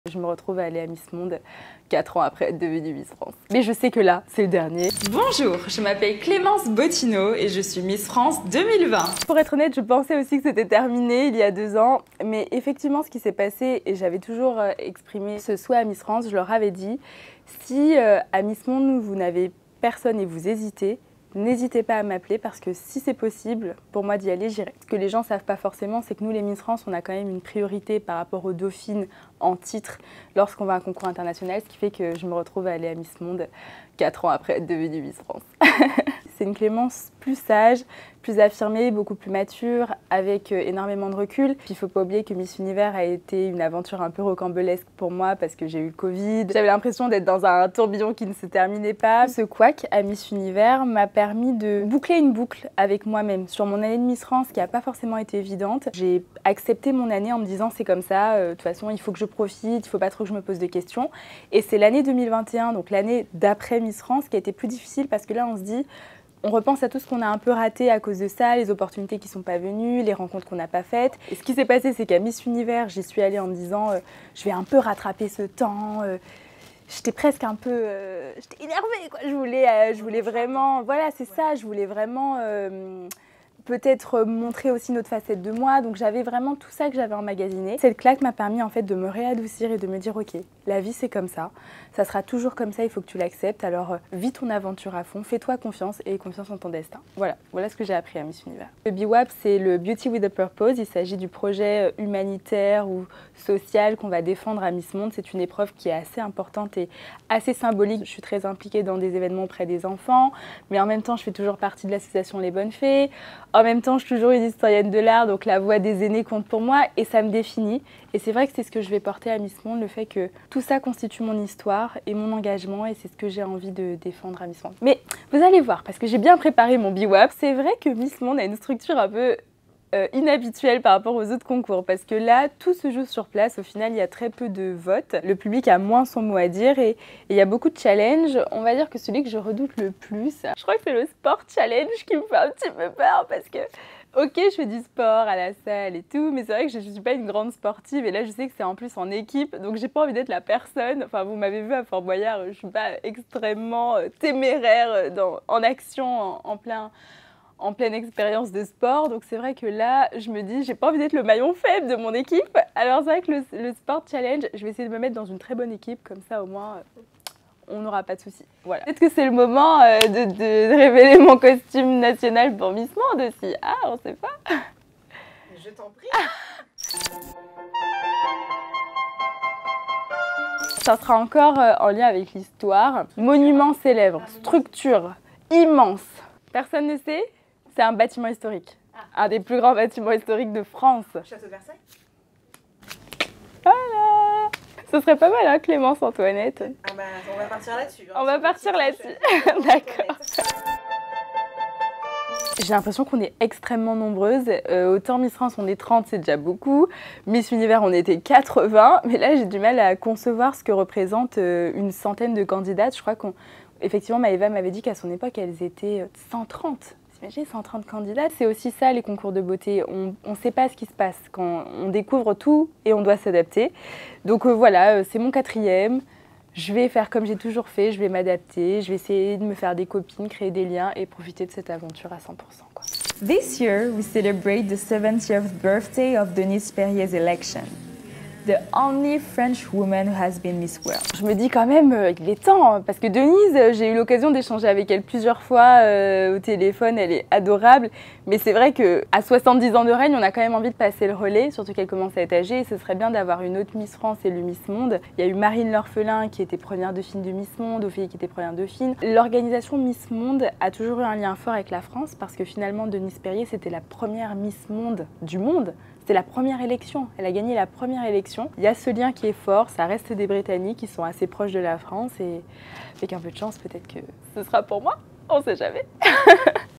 Clémence Botino à la conquête de Miss Monde !
Arrivée en Inde où aura lieu la finale du concours Miss Monde samedi 9 mars, Miss France 2020 est prête à mettre fin à 70 ans de disette pour le camp français. Une expérience pour laquelle elle se prépare depuis de longs mois, deux ans après avoir s’être classée dans le top 10 de Miss Univers.
TF1info a rencontré la Guadeloupéenne de 27 ans en novembre, avant que le concours ne soit reporté une énième fois.